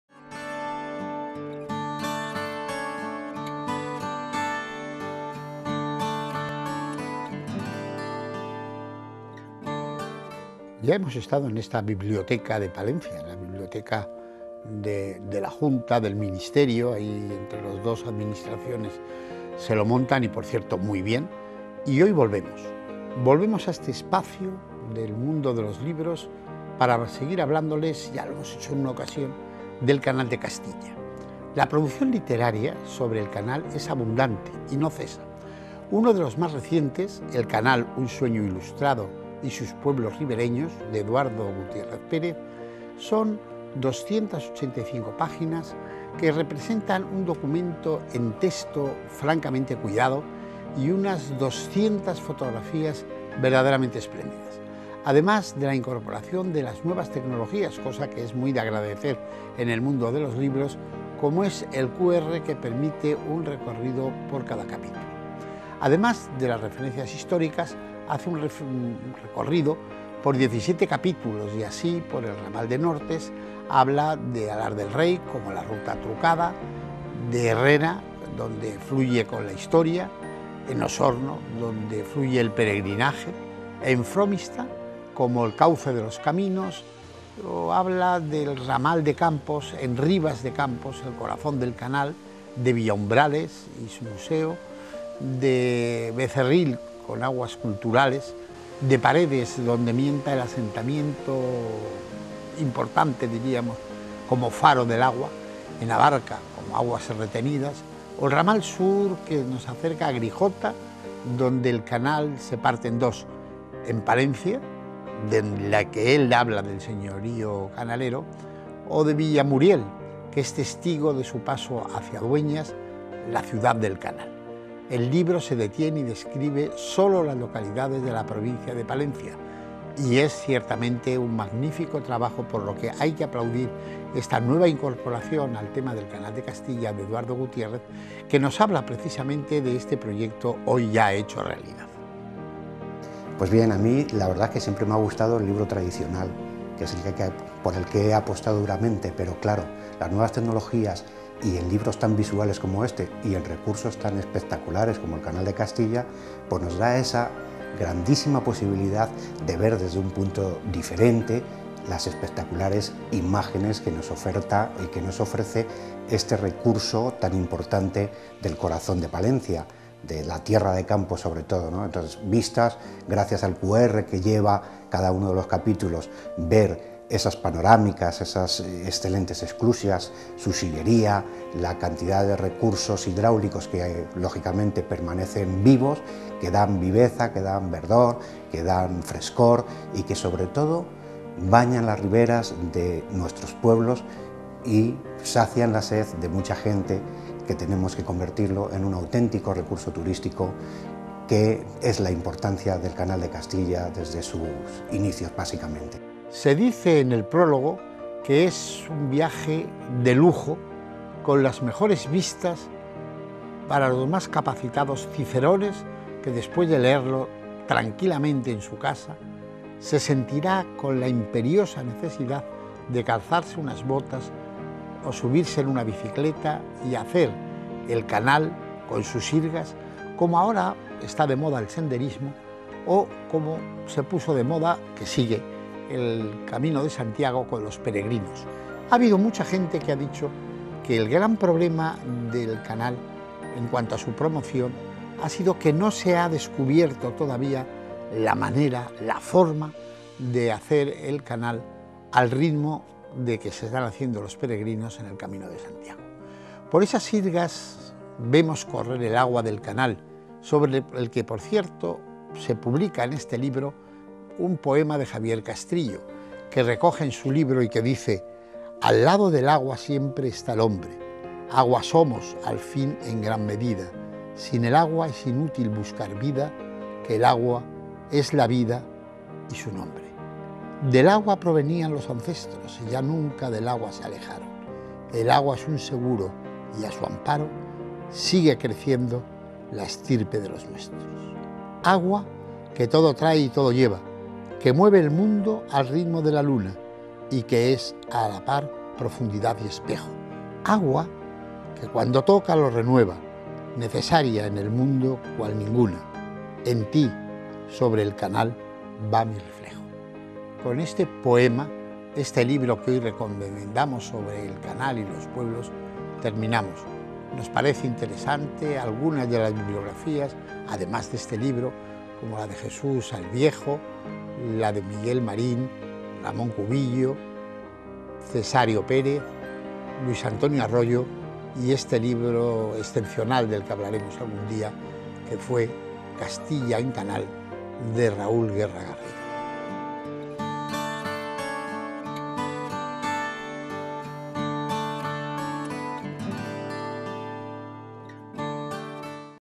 'Letras Palentinas' CANAL DE CASTILLA
En esta ocasión nos adentramos en el Canal de Castilla a través de distintas publicaciones, entre ellas "El Canal, el sueño ilustrado y sus pueblos ribereños" de Eduardo Gutiérrez Pérez.